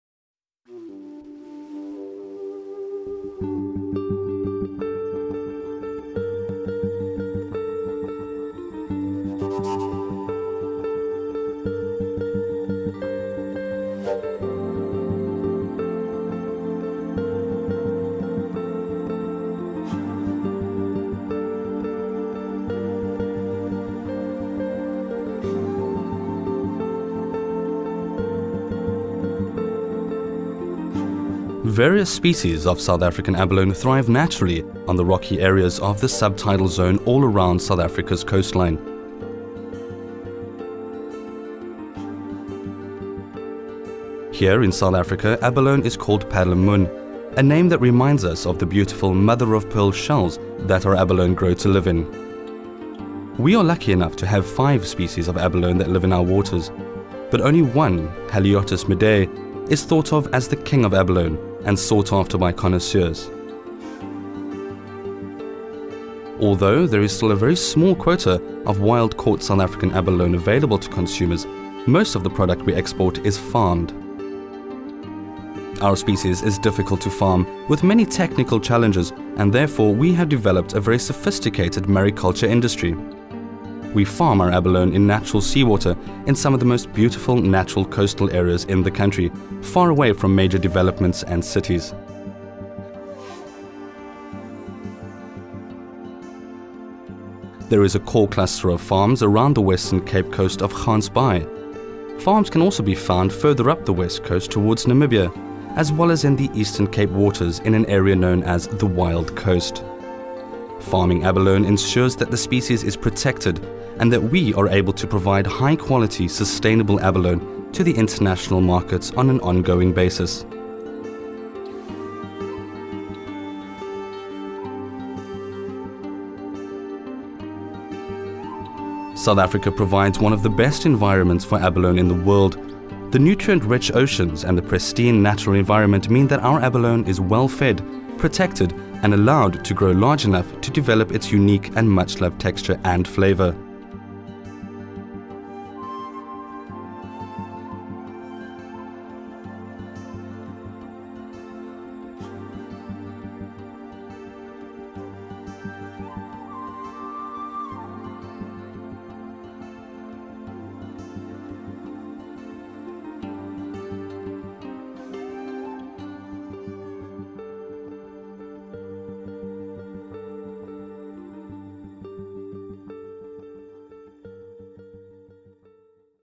South African Abalone
Haliotis midae is the much sought after South African species of molusc in the family Haliotidae -- commonly known as the abalones. In South Africa is it also colloquially called "perlemoen".

In nature abalone inhabits rocky surfaces in the subtidal zone all along the South African coast-line, but wild South African abalone is sadly quite threatened as a species. In order to ensure the survival of the species and continued supply to the global market, South African Abalone is farmed in fresh natural sea-water all along the coastal areas of the country.

South African abalone farming is the largest aquaculture industry in the country currently and supports communities in the pristine rural areas where the farms are located.